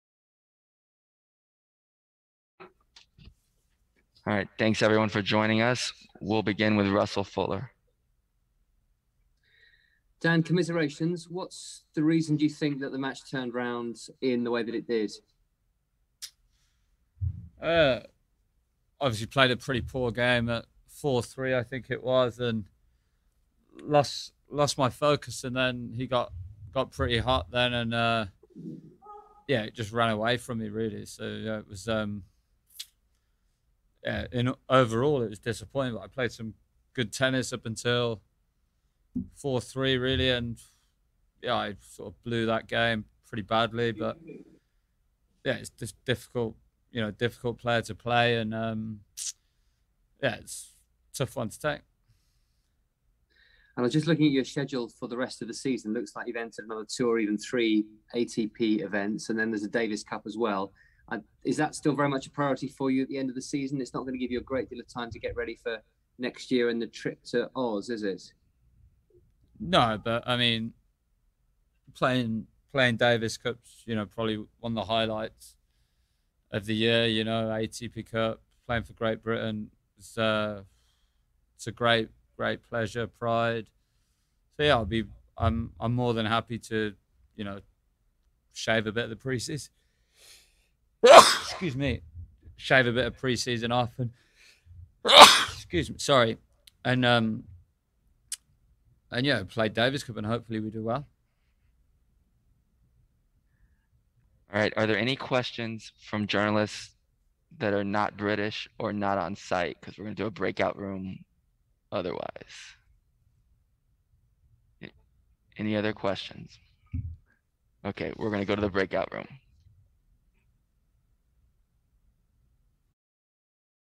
Daniel Evans Third Round Press Conference 2021 BNP Paribas Open
Daniel Evans meets with the media after his third round loss to Diego Schwartzman.